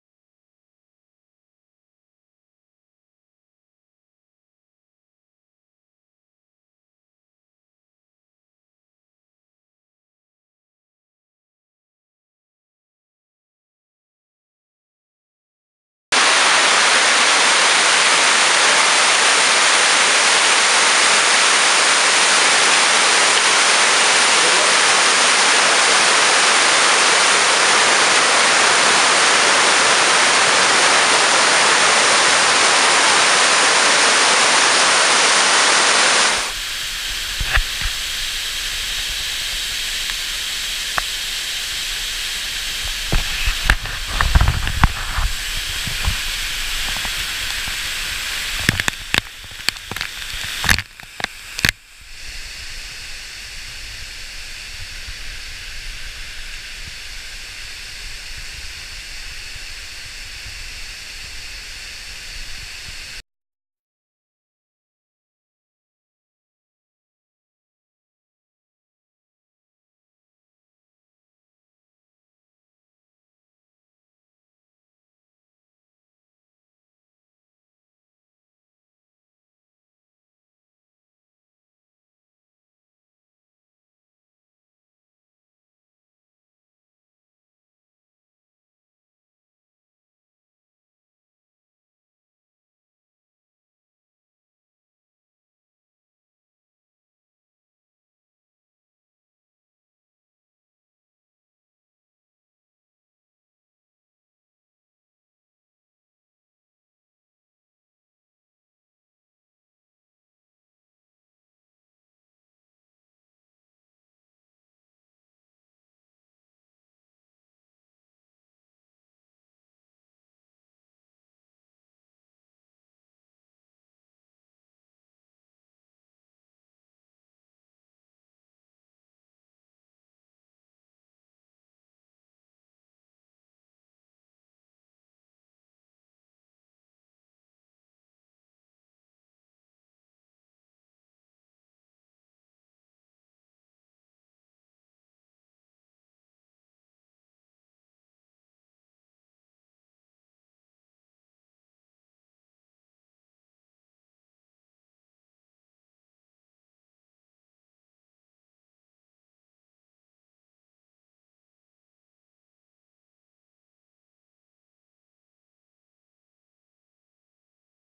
Derwent Dam In Overflow, 23/11/16
Derwent Dam in Overflow after Heavy Rainfall a few days ago, First over flow of Winter Filmed with Nikon D3100, GoPro Hero 4 Black and DJI Phantom 3 Advanced